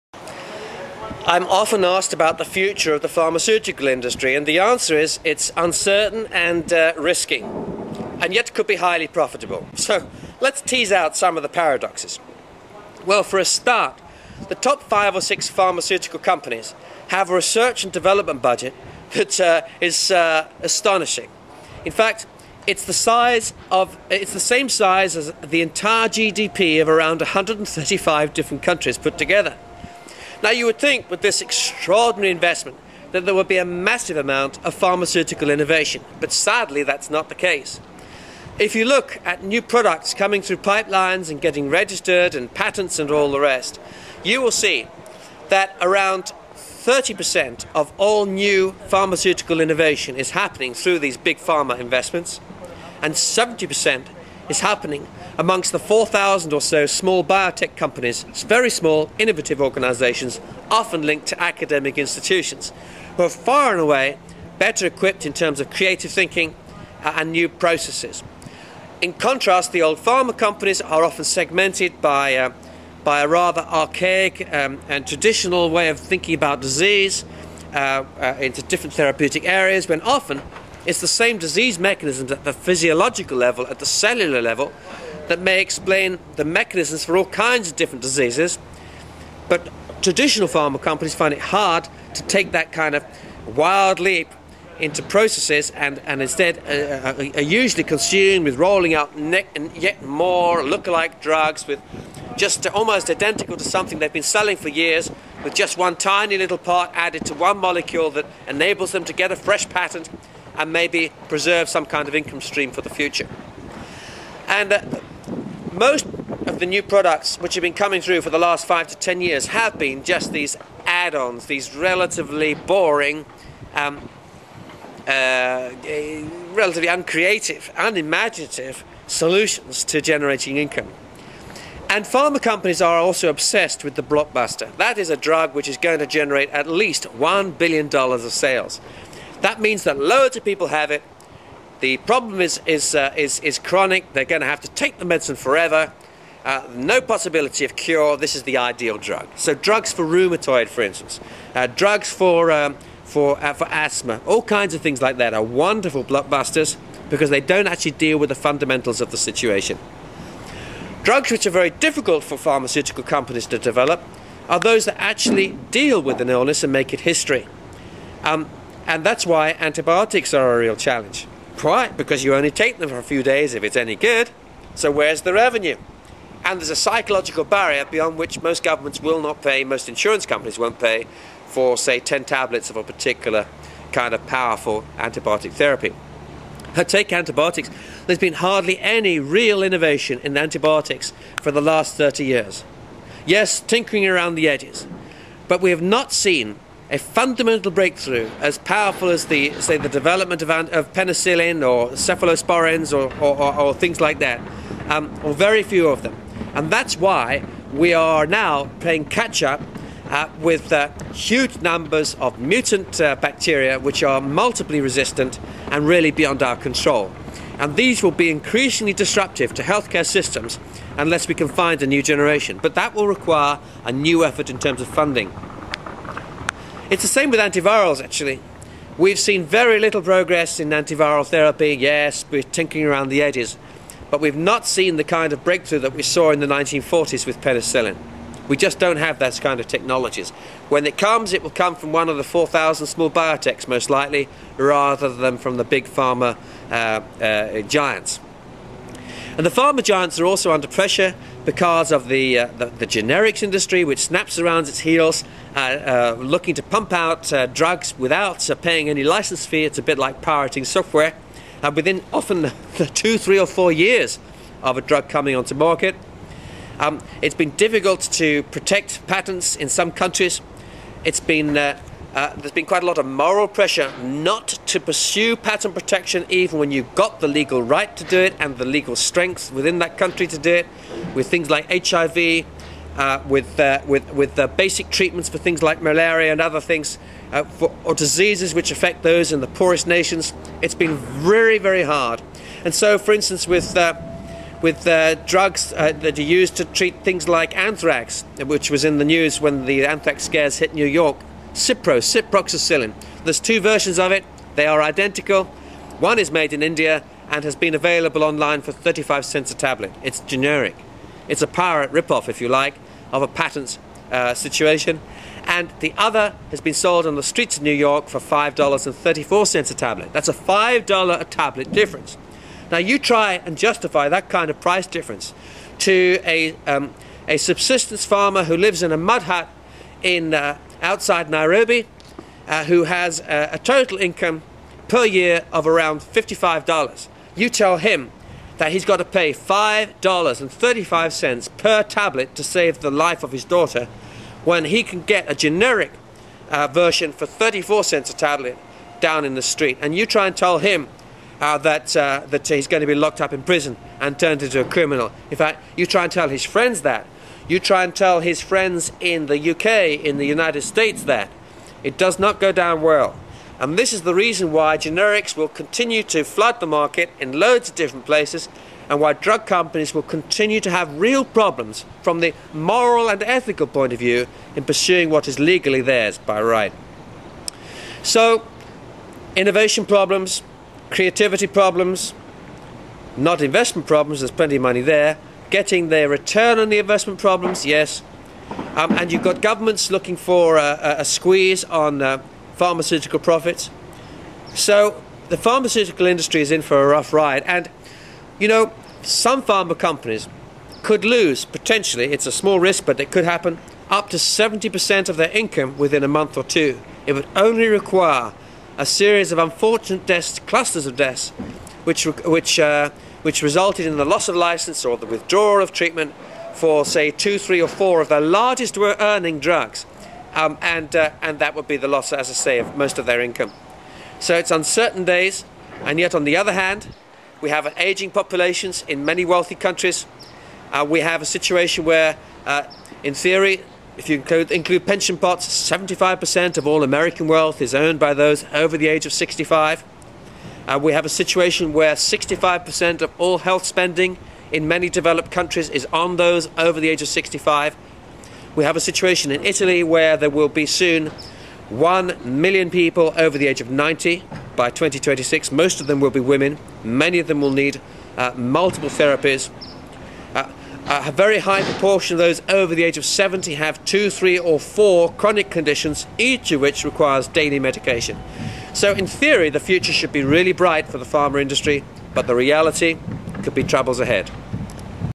Future of the pharmaceutical industry: new therapy, drug innovation, care and health keynote speaker
Lack of drug innovation and empty pharmaceutical product pipeline.  Generic manufacturer pharma competition and product recalls.  Patent expiry and intellectual property protection.  Legal challenges and research scandals.  Biogenerics and large molecule therapeutics.  Cellular mechanisms of disease.  Unmet health care needs.  Government purchasing policies and health insurance cover.  Online pharmacy price pressures.  Search for new health care treatment blockbusters.  Chronic disease and orphan drug therapies.  Rheumatoid arthritis and asthma.  Search for new antibiotics and multiple resistance infection problems.    Search for powerful antiviral therapy.  Future health funding and ageing populations.  Contrast with emerging nations.  Health treatment access and health injustice issues.  Pharmacogenomics and gene prophecy.  Ethical issues in gene therapy.  HIV and AIDS search for treatments, cure and vaccine.   Patrick Dixon, conference keynote speaker and futurist.